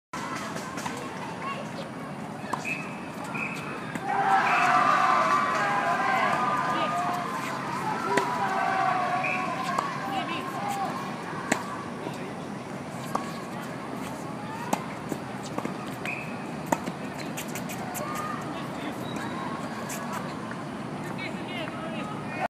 Jaeyoon Jung & Jac vs Louis Lin & Simin - video 2
Singapore Tennis Group.

The Avengers -  Mini Tennis Tournament for Amateur, mix double round 1. Team 'Taekwando Tennis' (Jaeyoon Jung & Jac) on the left  vs team 'I don't wan to miss a thing' (Louis Lin & Simin).

Date: 22/Jan/2013(Tuesday)
Time: 8pm

Both team play well !
Eventually Jaeyoon & Jac team won with close score 9-6